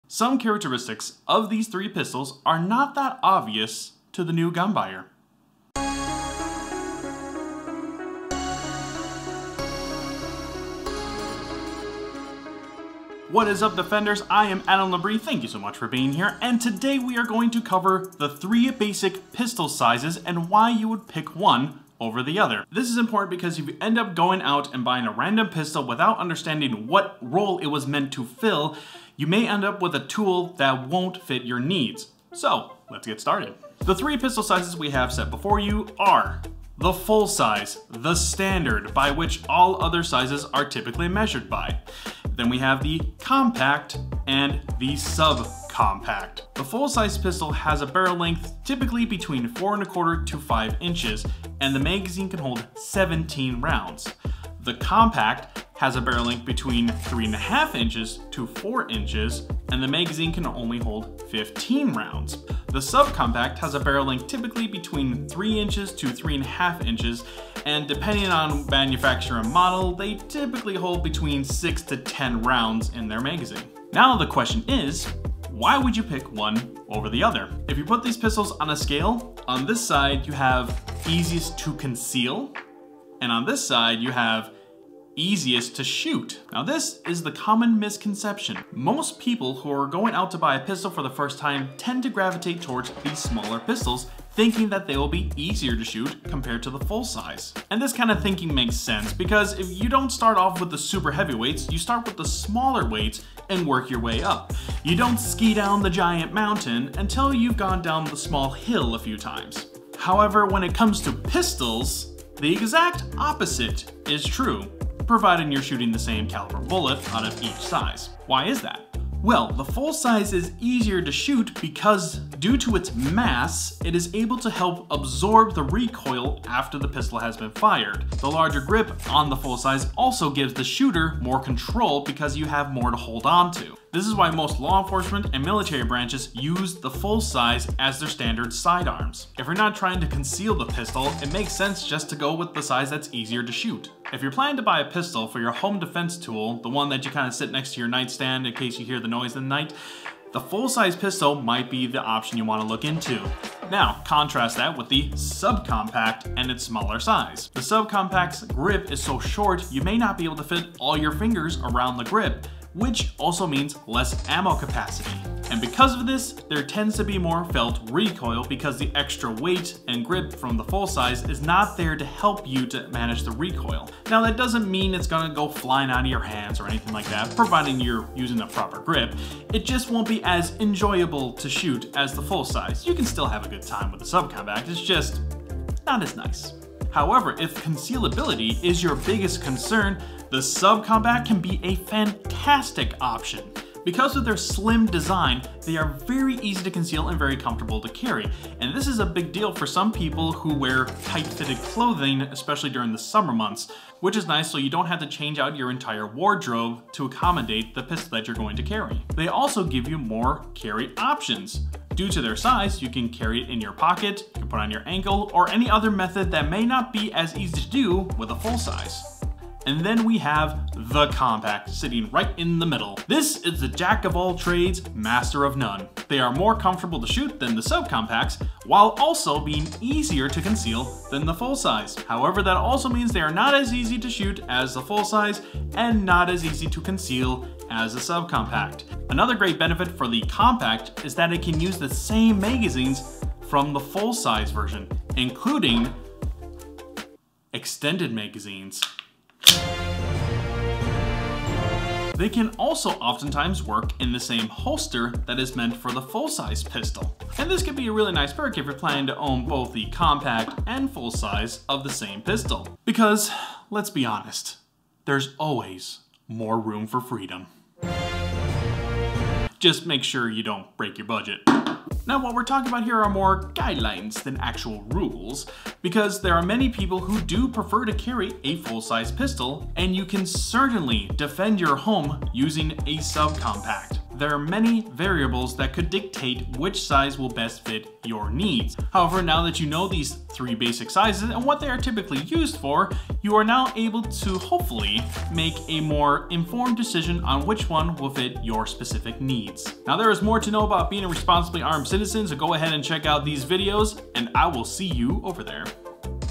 Pistol sizes explained: Full, Compact, and Sub-Compact
I explain what the three basic pistol sizes are and why you might pick one over the other.

Holsters:
Outside the waistband
Inside the waistband

Pepper spray:
POM Pepper spray pocket clip

POM Pepper spray hook

POM Pepper spray key chain loop